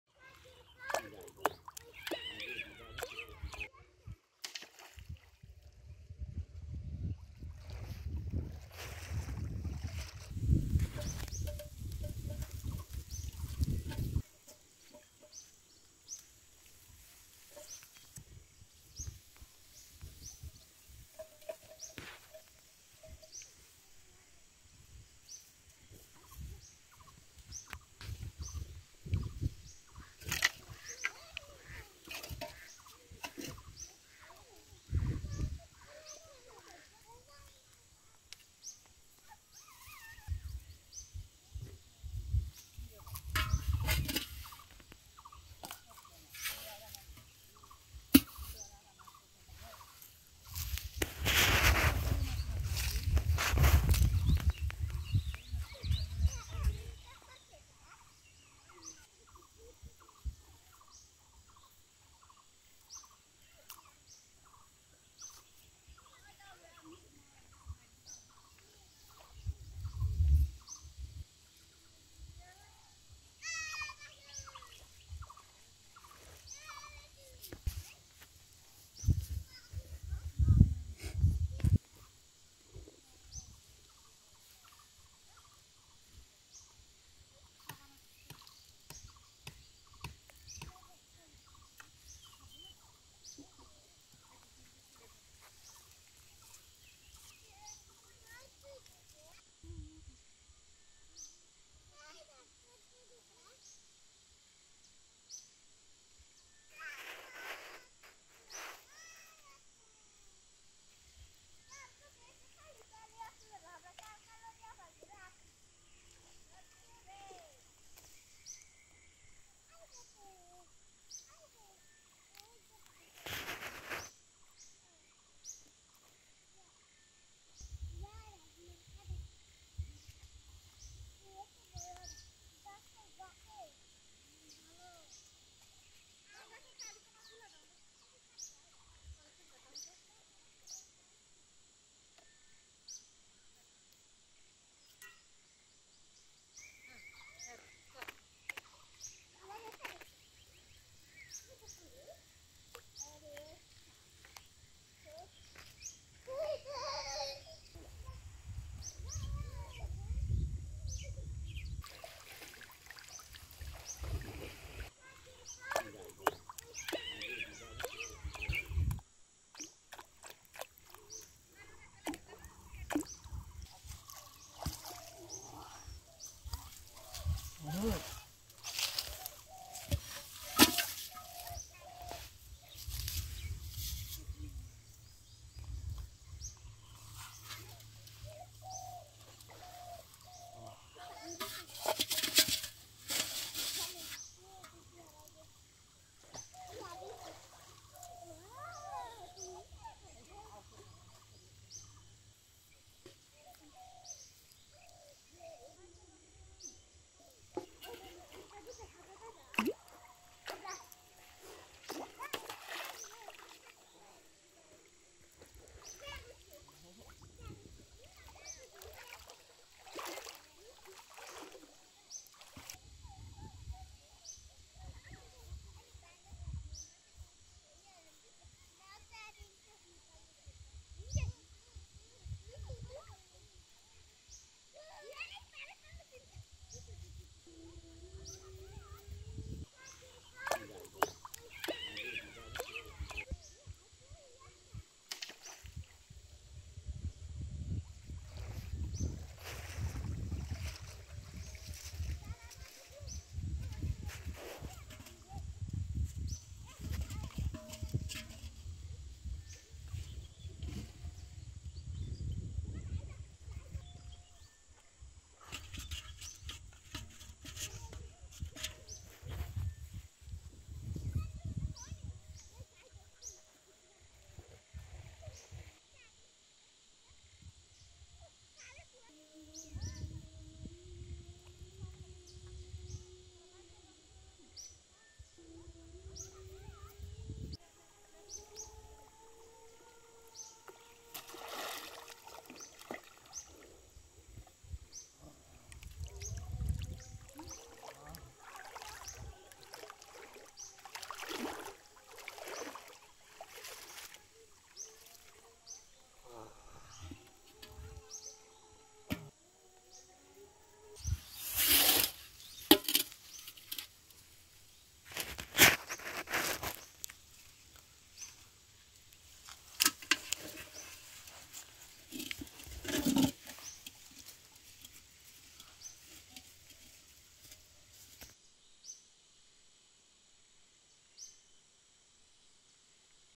Best Hook fishing 2022✅|Little Boy hunting fish by fish hook From beautiful nature🥰🥰Part-03
Best Hook fishing 2022✅|Little Boy hunting fish by fish hook From beautiful nature🥰🥰Part-02